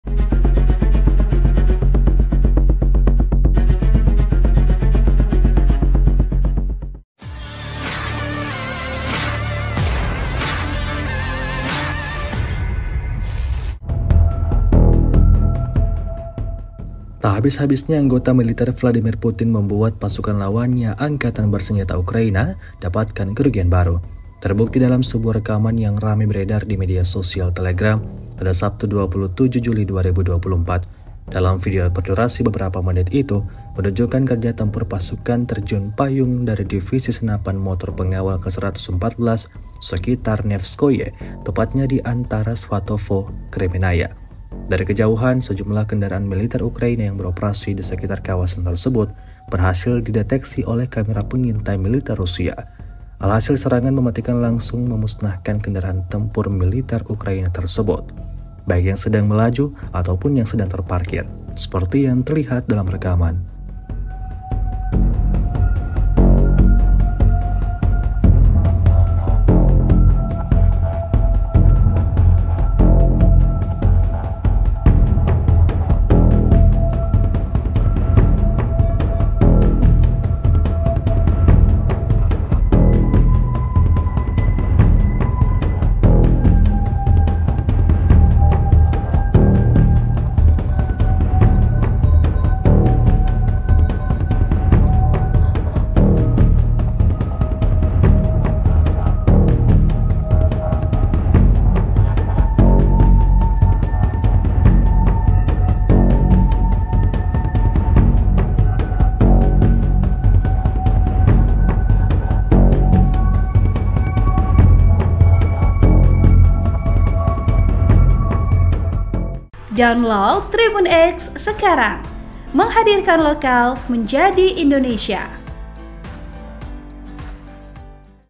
Identifikasi Kendaraan Musuhnya, Tentara Rusia Kerahkan Kamera Pengintai Profesional
TRIBUN-TIMUR.COM - Tak habis-habisnya anggota militer Vladimir Putin membuat pasukan lawannya, Angkatan Bersenjata Ukraina dapatkan kerugian baru.

Dalam video berdurasi beberapa menit itu menunjukkan kerja tempur pasukan terjun payung dari Divisi Senapan Motor Pengawal ke-144, sekitar Nevskoe, tepatnya di antara Svatovo-Kremennaya.

Dari kejauhan, sejumlah kendaraan militer Ukraina yang beroperasi di sekitar kawasan tersebut berhasil dideteksi oleh kamera pengintai militer Rusia.

Alhasil, serangan mematikan langsung memusnahkan kendaraan militer Ukraina tersebut.

Baik yang sedang melaju ataupun yang sedang terparkir.

Beredarnya video ini menuai beragam komentar dari warganet. (*)

Naskah: Memet Fani Gunawan
Narator: Memet Fani Gunawan
Editor Video: A. Syahrul Khair

(TRIBUN-TIMUR.COM)